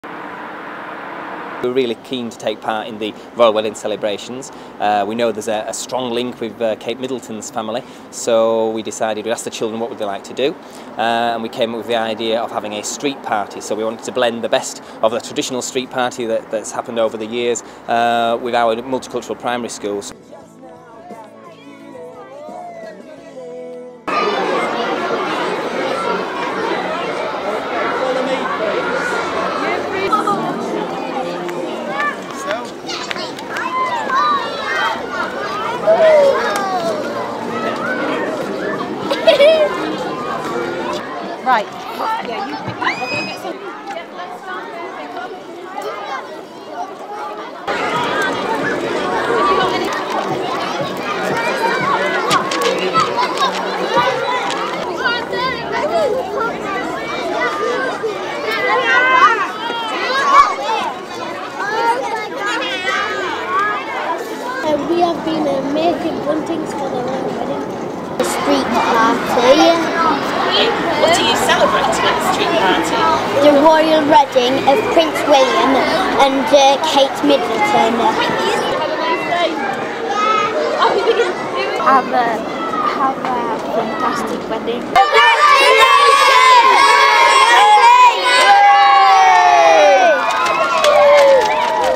Chapel Allerton, Leeds
Primary School Party

NAME: Catherine Henderson, Leeds Trinity University
LOCATION RECORDED: Chapel Allerton, Leeds